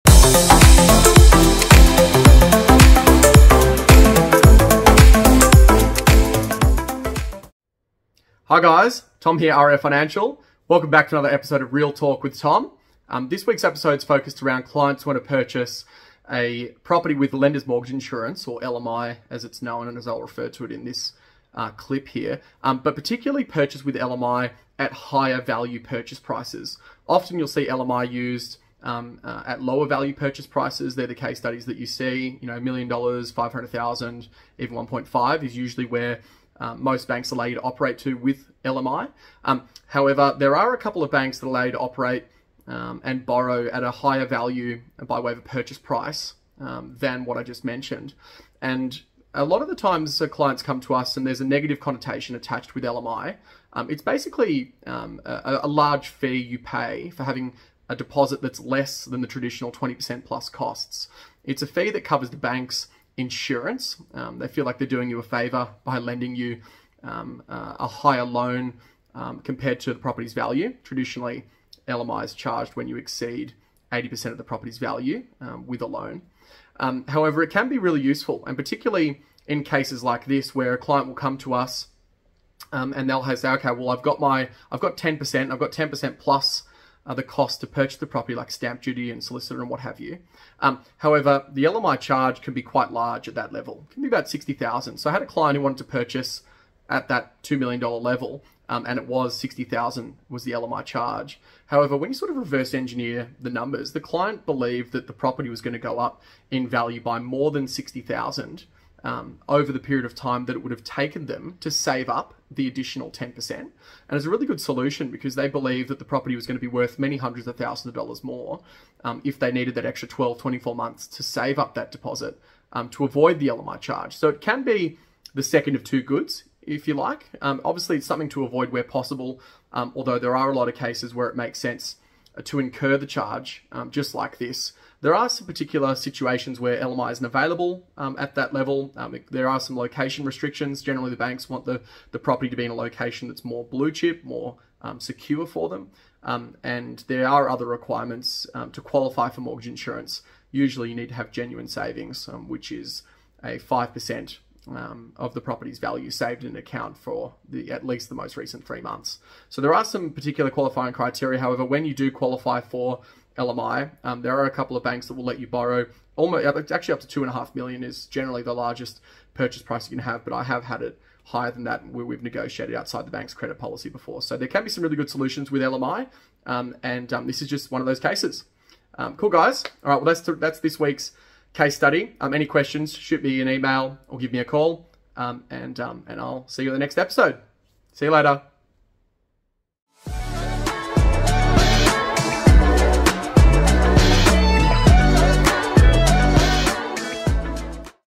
EP: 2 | Reel Talk with Tom
Tom talks about how Lenders Mortgage Insurance or LMI works.